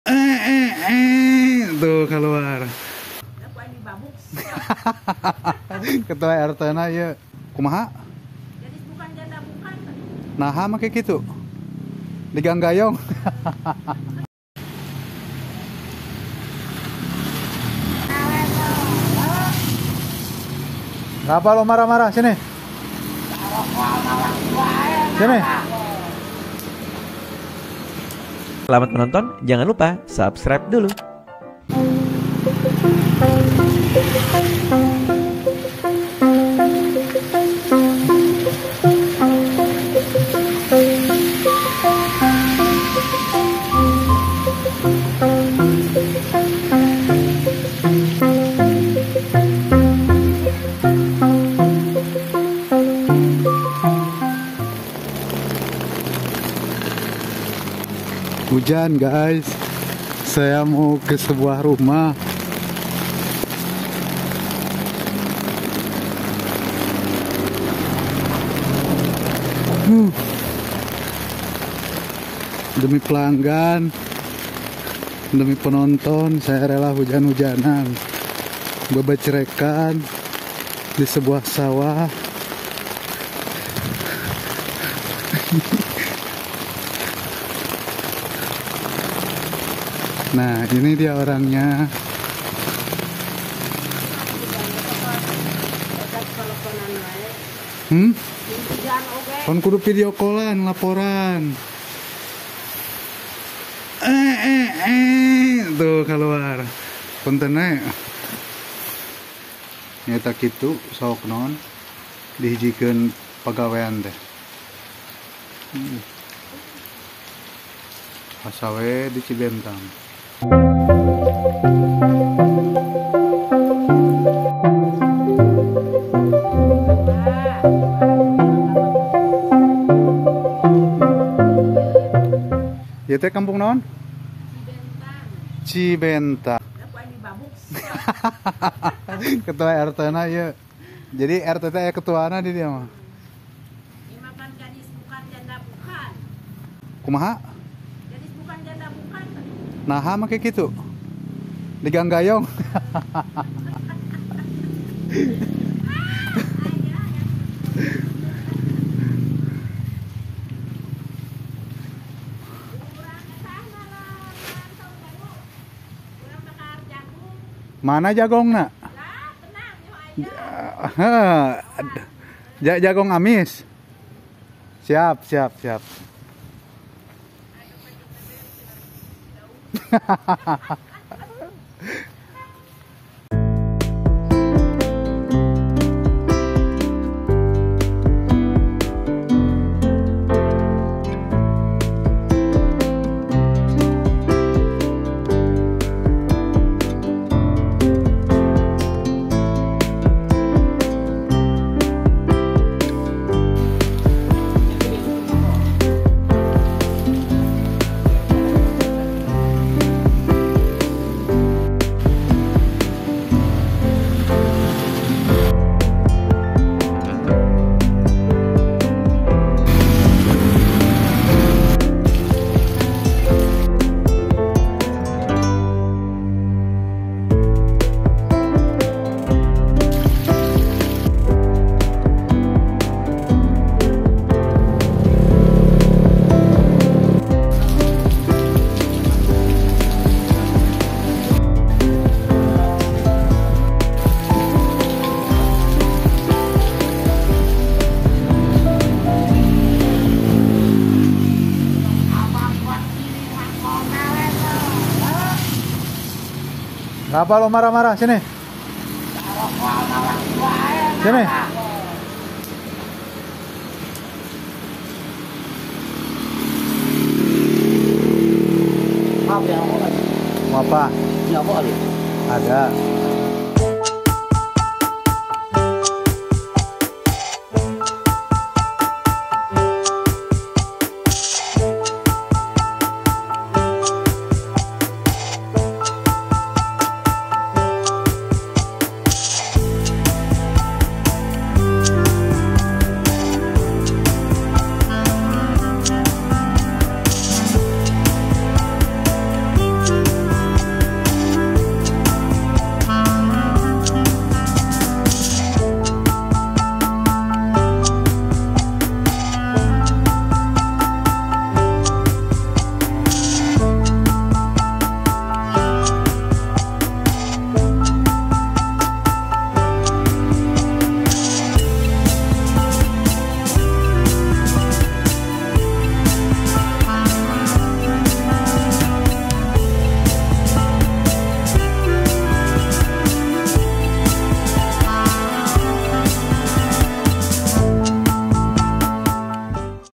Hujan-Hujanan Nganterin Pesanan Ke Rumah Janda, Ketemu ODGJ Marah-Marah Di Jalan, Suasana Pedesaan
Suasana pedesaan Hujan Deras Mengguyur Kecamatan Sindangbarang Cianjur Selatan, Hujan Hujanan ke rumah janda melewati sawah yang indah, dan ketemu odgj marah marah karena lapar dan ingin merokok.